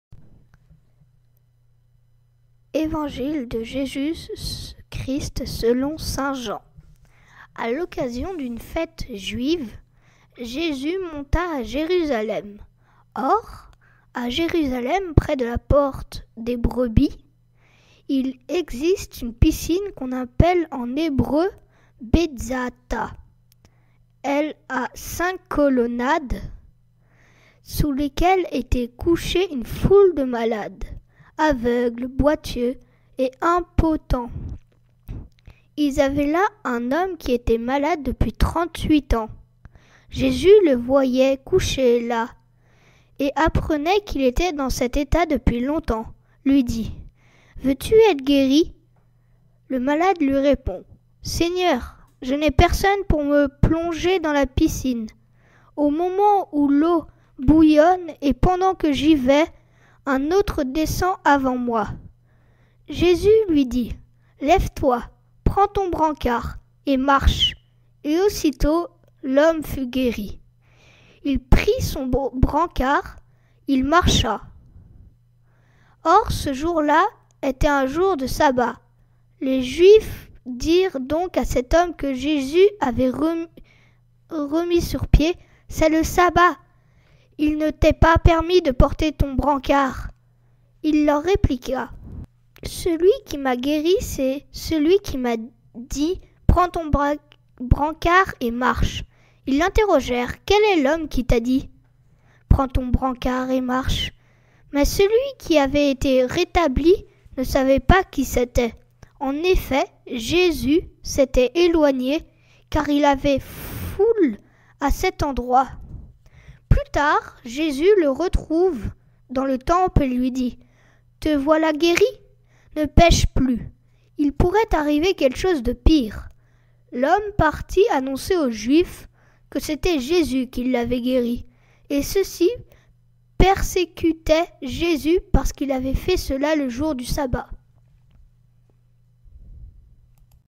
Évangile 24 mars 2020 /« Aussitôt l’homme fut guéri » (Jn 5, 1-16)
À l’occasion d’une fête juive,
Jésus monta à Jérusalem.
Or, à Jérusalem, près de la porte des Brebis,
il existe une piscine qu’on appelle en hébreu Bethzatha.
Elle a cinq colonnades,
sous lesquelles étaient couchés une foule de malades,
aveugles, boiteux et impotents.
Il y avait là un homme qui était malade depuis trente-huit ans.
Jésus, le voyant couché là,
et apprenant qu’il était dans cet état depuis longtemps,
lui dit :
« Veux-tu être guéri ? »
Le malade lui répondit :
« Seigneur, je n’ai personne
pour me plonger dans la piscine
au moment où l’eau bouillonne ;
et pendant que j’y vais,
un autre descend avant moi. »
Jésus lui dit :
« Lève-toi, prends ton brancard, et marche. »
Et aussitôt l’homme fut guéri.
Il prit son brancard : il marchait !
Or, ce jour-là était un jour de sabbat.
Les Juifs dirent donc à cet homme que Jésus avait remis sur pied :
« C’est le sabbat !
Il ne t’est pas permis de porter ton brancard. »
Il leur répliqua :
« Celui qui m’a guéri, c’est lui qui m’a dit :
“Prends ton brancard, et marche !” »
Ils l’interrogèrent :
« Quel est l’homme qui t’a dit :
Mais celui qui avait été rétabli
ne savait pas qui c’était ;
en effet, Jésus s’était éloigné,
car il y avait foule à cet endroit.

Plus tard, Jésus le retrouve dans le Temple et lui dit :
« Te voilà guéri.
Ne pèche plus,
il pourrait t’arriver quelque chose de pire. »
L’homme partit annoncer aux Juifs
que c’était Jésus qui l’avait guéri.
Et ceux-ci persécutaient Jésus
parce qu’il avait fait cela le jour du sabbat.